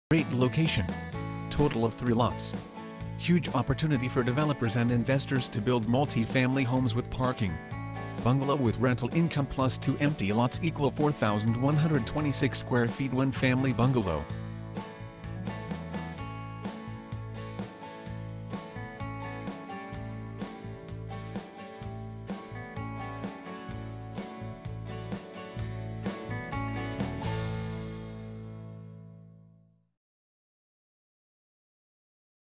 2822 Brown St Brooklyn NY
Great location! Total of 3 lots  Huge opportunity for developers and investors to build multifamily homes with parking! Bungalow with rental income + 2 empty  lots =
4,126 square feet 1 family bungalow

Video uploaded on Fri 11 May, 2012
real estate Brooklyn NY